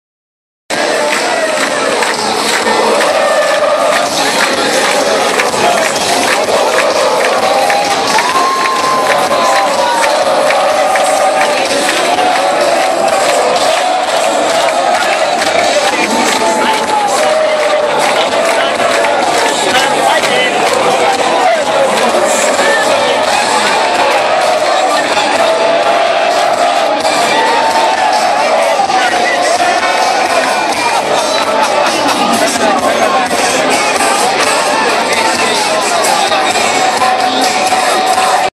Swedish corner
swedish having fun in kiev fan zone